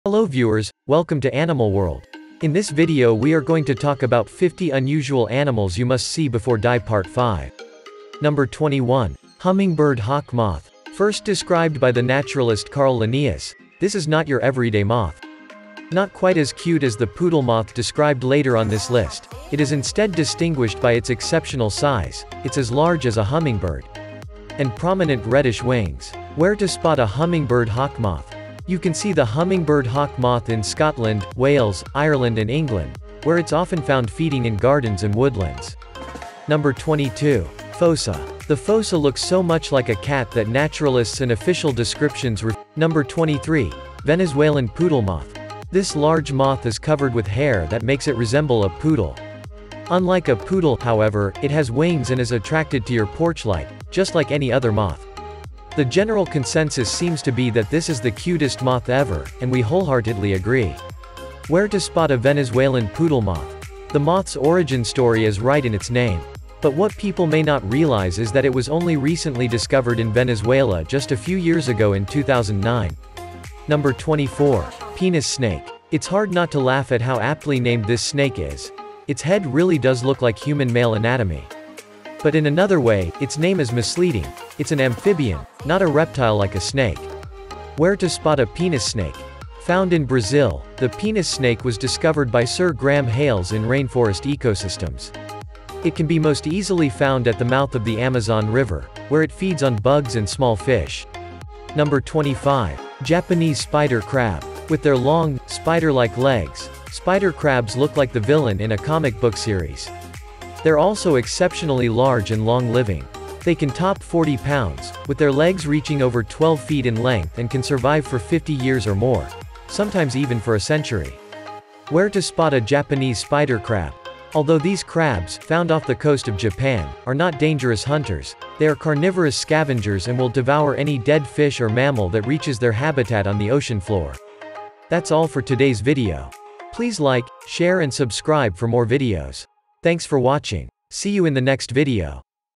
Most UNUSUAL ANIMALS on Earth | Part 5
Hello viewers, Welcome to animal world. In this video we are going to talk about 50 Unusual Animals you must see before die Part 5.
Number 21. Hummingbird Hawk-Moth.
First described by the naturalist Carl Linnaeus, this is not your everyday moth.

Not quite as cute as the poodle moth described later on this list, it is instead distinguished by its exceptional size — it’s as large as a hummingbird! — and prominent reddish wings.
Where to Spot a Hummingbird Hawk-Moth.
You can see the hummingbird hawk-moth in Scotland, Wales, Ireland and England, where it’s often found feeding in gardens and woodlands.
Number 22. Fossa.
The fossa looks so much like a cat that naturalists and official descriptions refer to it as “cat-like” — even though it’s actually a member of the weasel family and is a close relative of the mongoose.

While this remarkable creature does have the ears and head shape of a cat, it boasts one totally unique feature: a long tail that it uses to grip tree branches as it climbs in search of food.
Where to Spot a Fossa.
Like many odd and unusual animals, the fossa calls Madagascar home.

As the largest predator on the island nation, it feasts on everything from rodents to lemurs. Its range is also exceptional, covering everything from the lowlands to the plateaus.
Number 23.Venezuelan Poodle Moth.
This large moth is covered with hair that makes it resemble a poodle. Unlike a poodle, however, it has wings and is attracted to your porch light, just like any other moth.

The general consensus seems to be that this is the cutest moth ever, and we wholeheartedly agree.
Where to Spot a Venezuelan Poodle Moth.
The moth's origin story is right in its name.

But what people may not realize is that it was only recently discoverd in Venezuela just a few years ago in 2009.
Number 24. Penis Snake.
It’s hard not to laugh at how aptly named this snake is; its head really does look like human male anatomy.

But in another way, its name is misleading: It’s an amphibian, not a reptile like a snake.
Where to Spot a Penis Snake.
Found in Brazil, the penis snake was discovered by Sir Graham Hales in rainforest ecosystems.

It can be most easily found at the mouth of the Amazon River, where it feeds on bugs and small fish.
Number 25. Japanese Spider Crab.
With their long, spider-like legs, spider crabs look like the villain in a comic-book series. They’re also exceptionally large and long-living.

They can top 40 pounds, with their legs reaching over 12 feet in length and can survive for 50 years or more ... sometimes even for a century!
Where to Spot a Japanese Spider Crab.
Although these crabs, found off the coast of Japan, are not dangerous hunters, they are carnivorous scavengers and will devour any dead fish or mammal that reaches their habitat on the ocean floor.